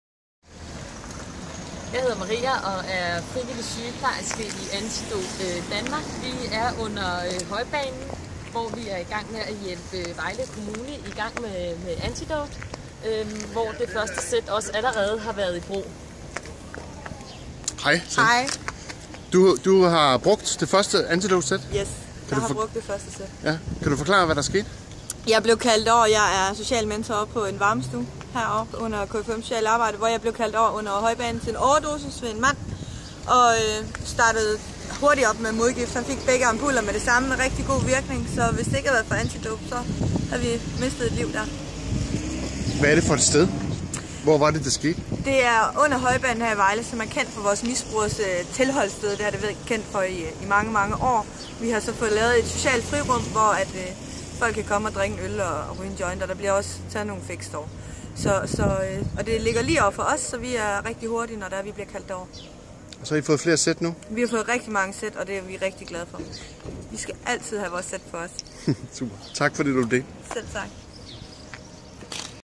"Sådan brugte jeg modgift"
Vejle Kommune har med hjælp fra Antidote Danmark startet Antidote op. Sanne Bjergaard fra KFUM's Paraplyen i Vejle fortæller her hvordan det første modgiftssæt blev brug under Højbanen.